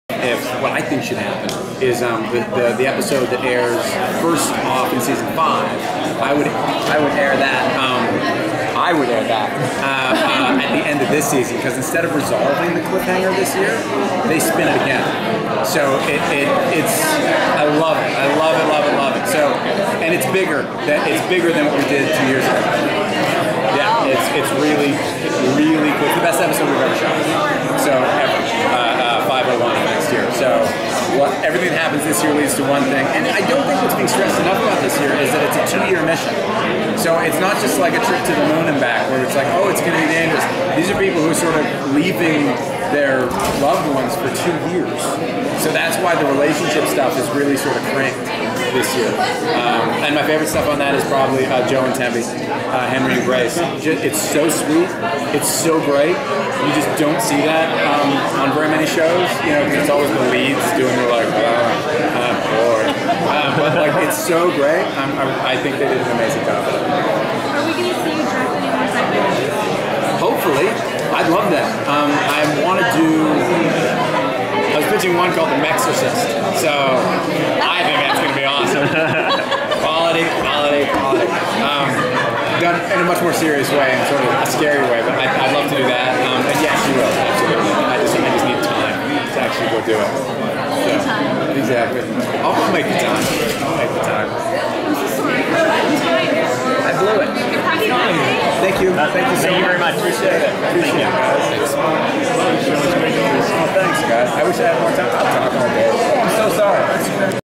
Colin Ferguson discussing Eureka in press only room at San Diego Comic Con
SyFy Channel hosted a press room for Eureka at San Diego Comic Con and Metal Life Magazine was there. Here is a clip of Colin Ferguson during the interviews.